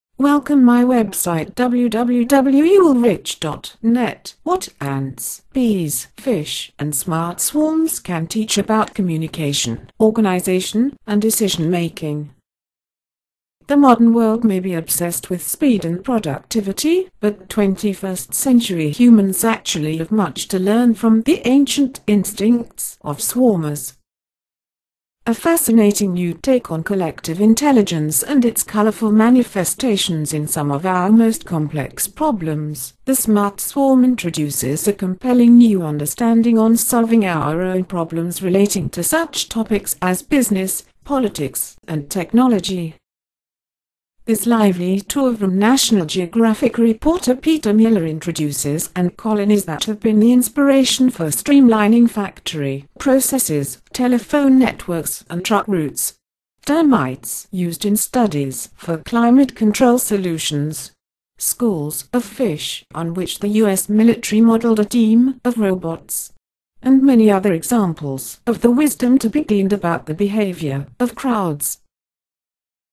The Smart Swarm
What ants, bees, fish, and smart swarms can teach about communication, organization, and decision-making.

The modern world may be obsessed with speed and productivity, but twenty-first-century humans actually have much to learn from the ancient instincts of swarmers. A fascinating new take on collective intelligence and its colorful manifestations in some of our most complex problems, The Smart Swarm introduces a compelling new understanding on solving our own problems relating to such topics as business, politics, and technology.

This lively tour from National Geographic reporter Peter Miller introduces ant colonies that have been the inspiration for streamlining factory processes, telephone networks, and truck routes; termites, used in studies for climate-control solutions; schools of fish, on which the U.S. military modeled a team of robots; and many other examples of the wisdom to be gleaned about the behavior of crowds.